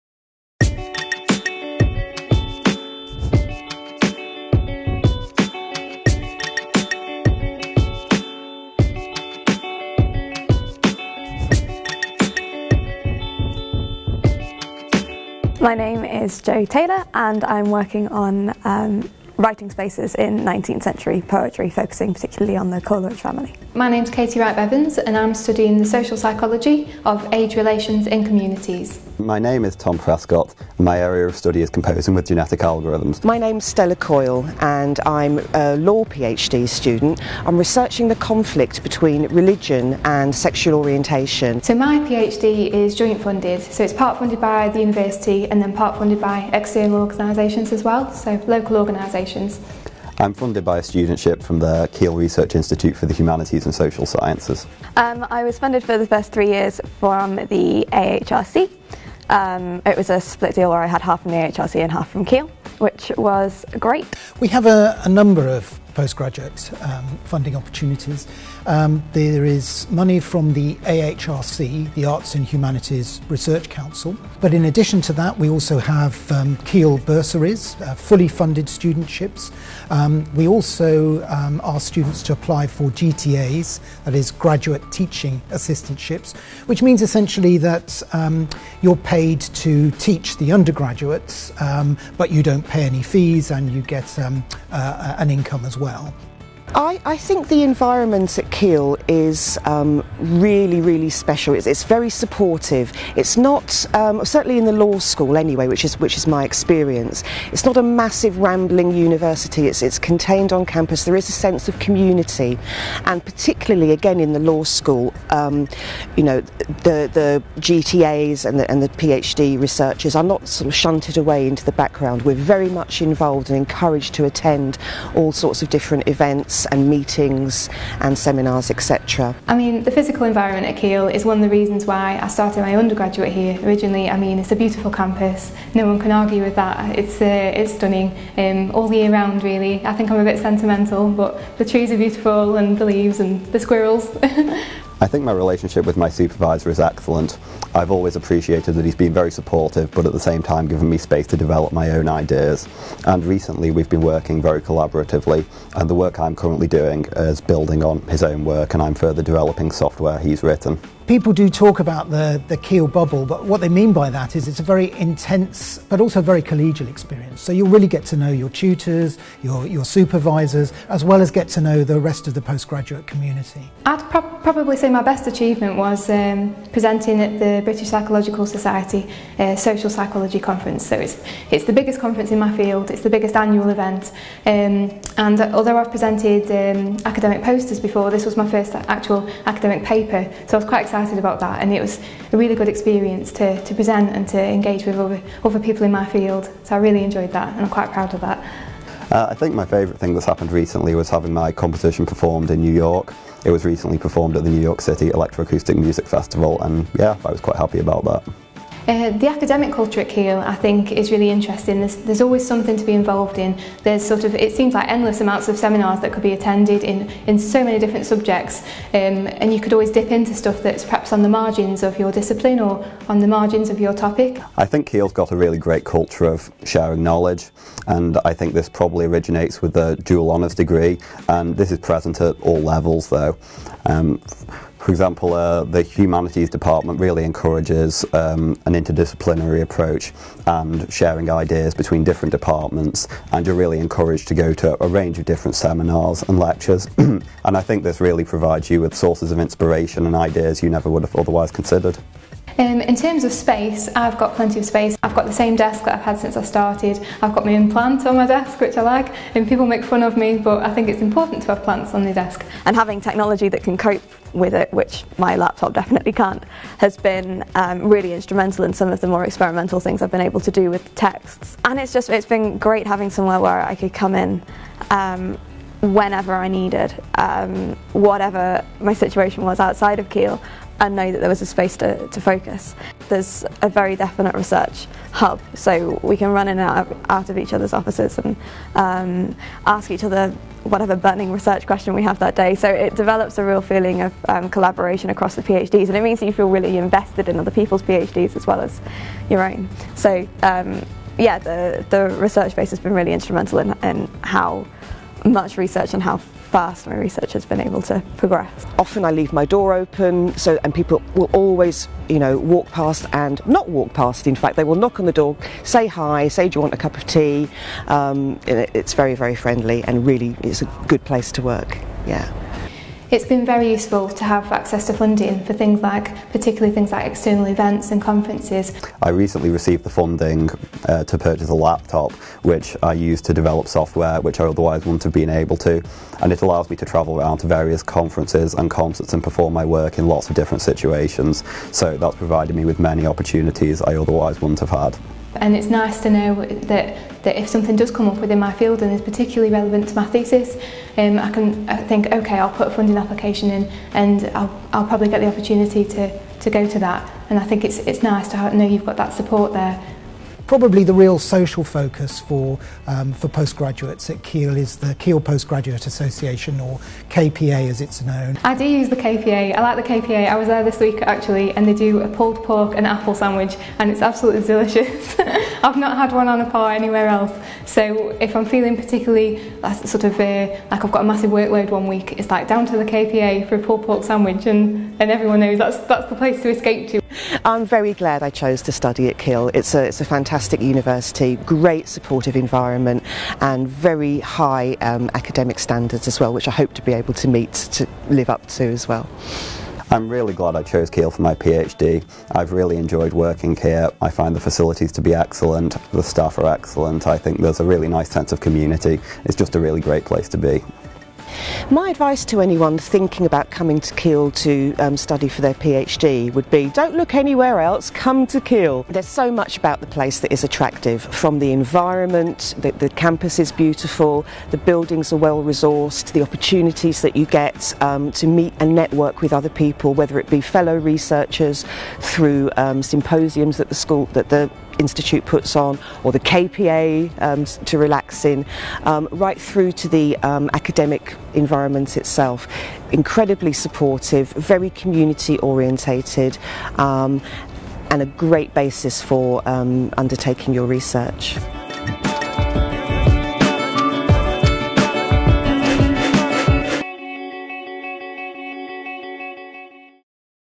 Humanities and Social Sciences PhD
Get an overview of Humanities and Social Sciences Postgraduate Research at Keele University.  Hear what our students think about life at Keele, our faculties and our staff.